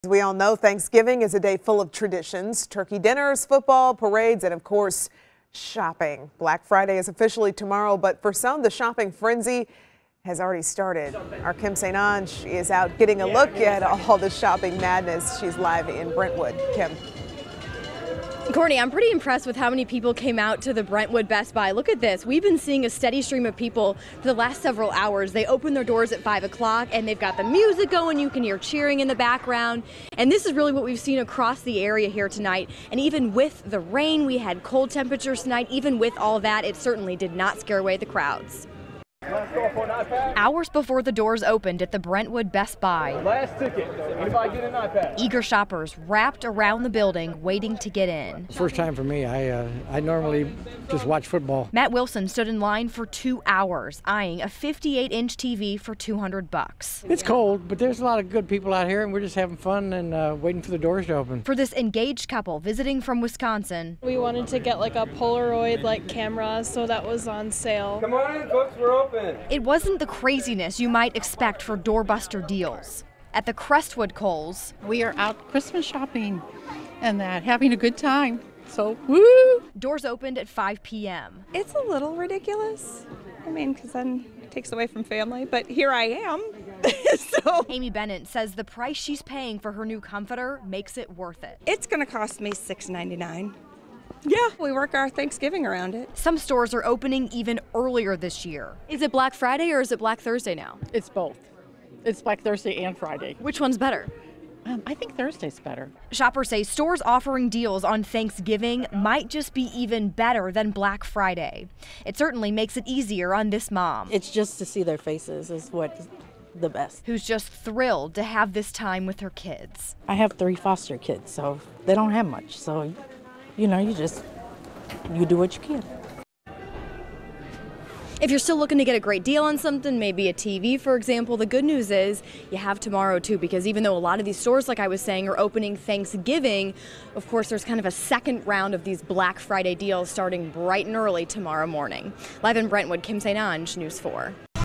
Shoppers pack local stores on Thanksgiving night
Stores across the St. Louis area were busy Thursday night as shoppers waited in long lines to get doorbuster deals.